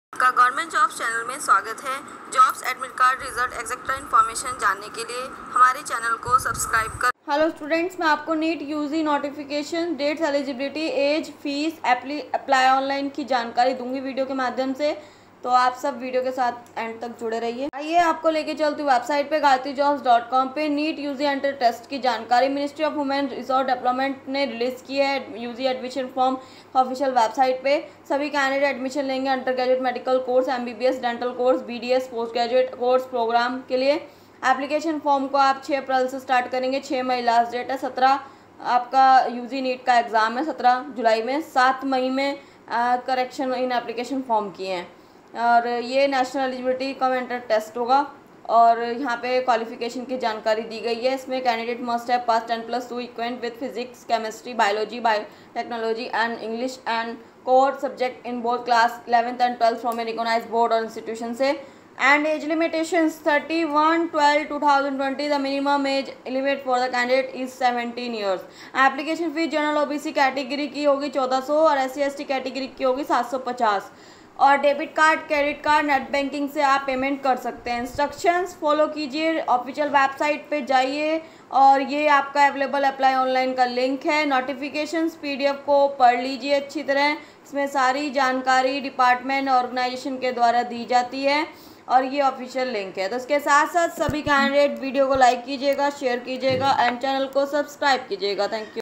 NEET UG Notification 2022 Dates Eligibility Age Fee Apply Online Link Activated
Ministry of Human Resource Development has released the NEET UG Notification on its official website.
NEET UG Registration starting date - 06/04/2022
NEET UG Online Registration Closing Date - 06/05/2022
Dates of UG NEET Examination - 17/07/2022
Candidates who want to take admission in Undergraduate Medical Course (MBBS), Dental Course (BDS) or postgraduate course Programme then they may fill the NEET UG application form from the official website of the department available